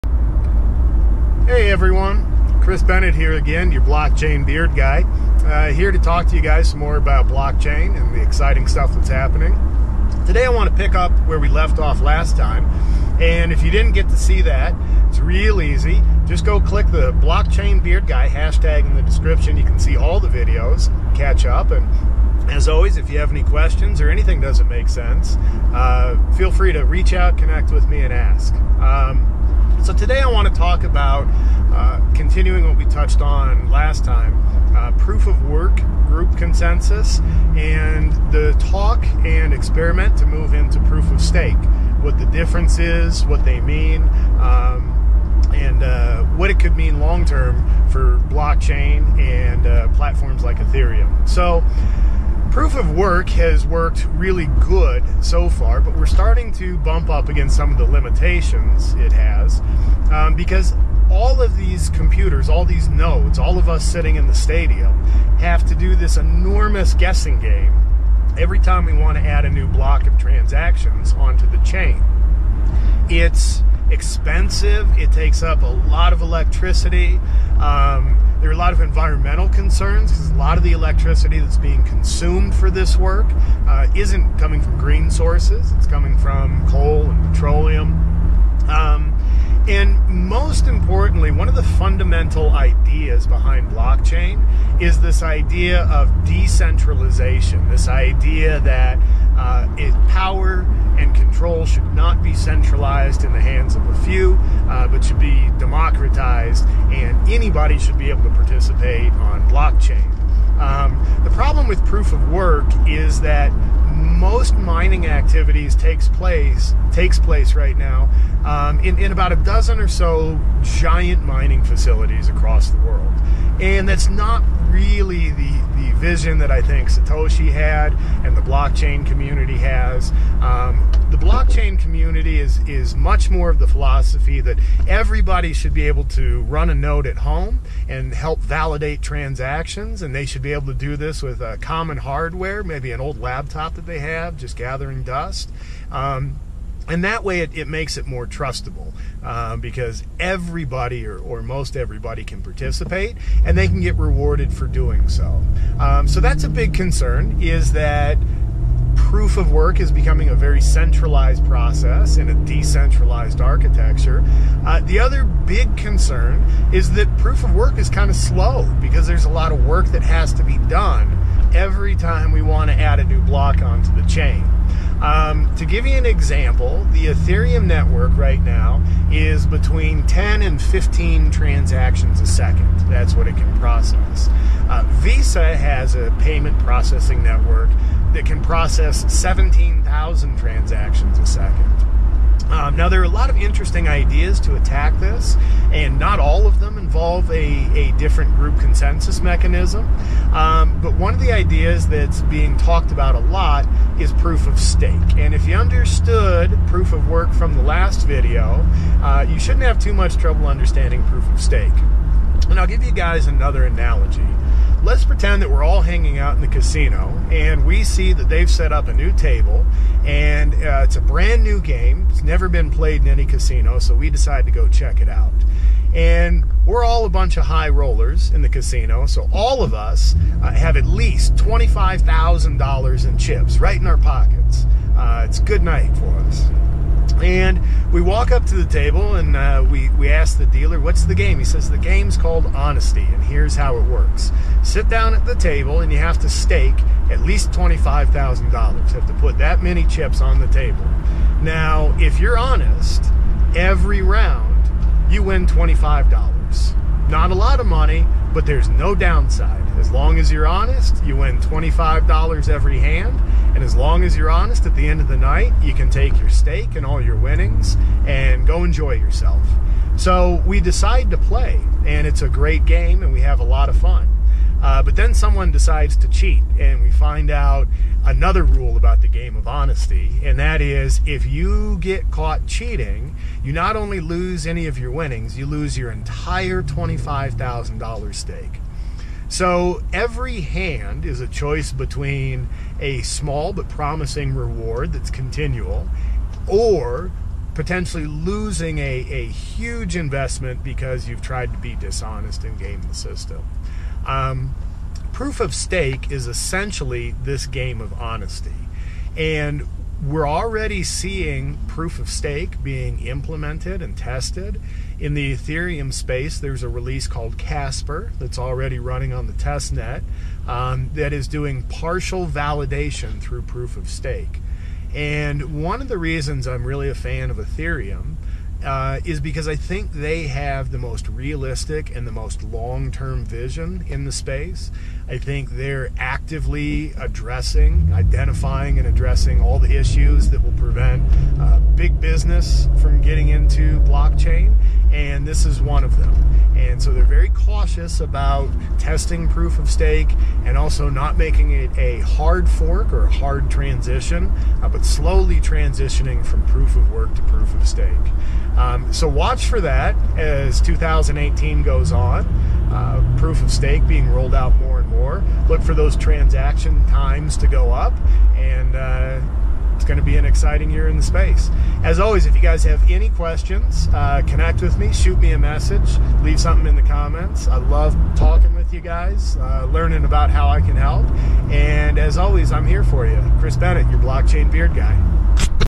Blockchain Beard Guy - What is Proof of Stake?
It's time for episode 4 of BlockchainBeardGuy!

Today we pick up where we left off yesterday and talk about Proof of Stake. What is it? How is it different from Proof of Work? Most importantly, what will all this mean in the -real- world?